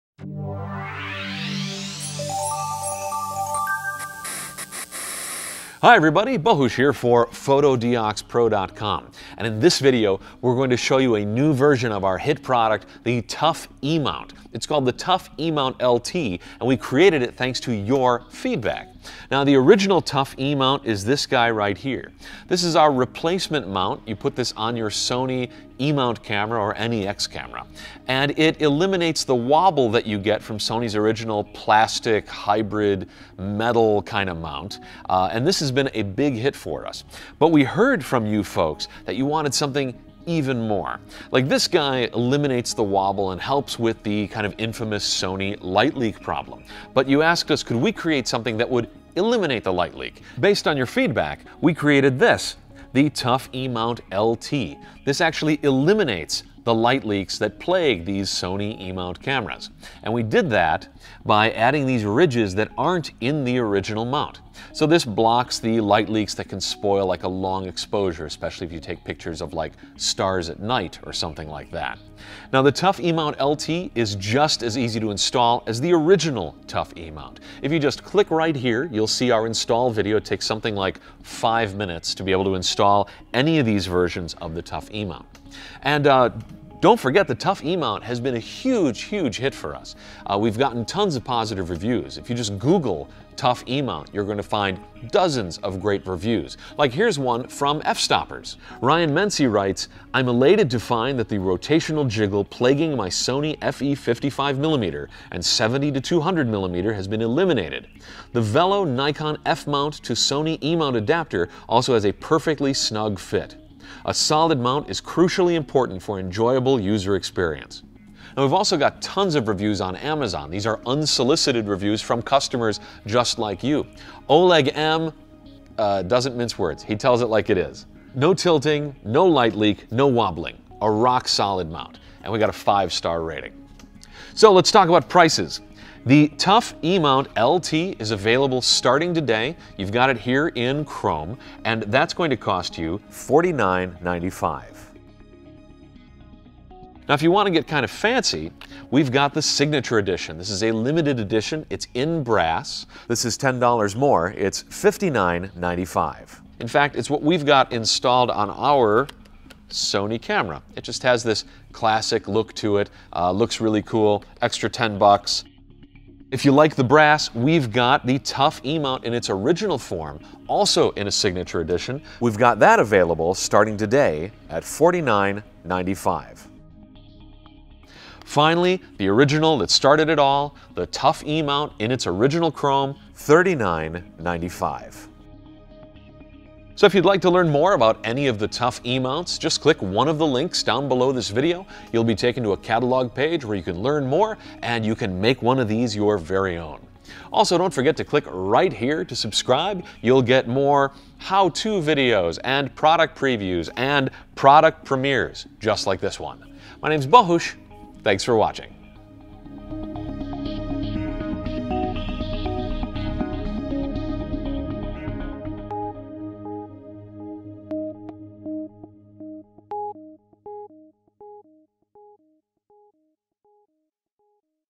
TOUGH E-Mount LT for Sony E-Mount Cameras - Eliminate Light Leaks and Lens Wobble
Last fall we introduced the TOUGH E-Mount, a simple kit that eliminates the lens wobble on your Sony E-mount cameras. We were amazed by the response (see what Fstoppers, PetaPixel, DPReview and SLR Lounge had to say), but your feedback was the most important to us. Because of your requests we’ve created something we’re really excited about: the all-new TOUGH E-Mount LT.

Available in chrome and Signature Edition brass, the TOUGH E-Mount LT incorporates extra flanges that eliminate the original E-mount’s notorious light leak problem. In only 5 minutes, you can replace your camera’s original two-piece E-mount with the LT’s one-piece, all-metal mount, locking your lens securely onto your camera and leaving your images streak and glare-free.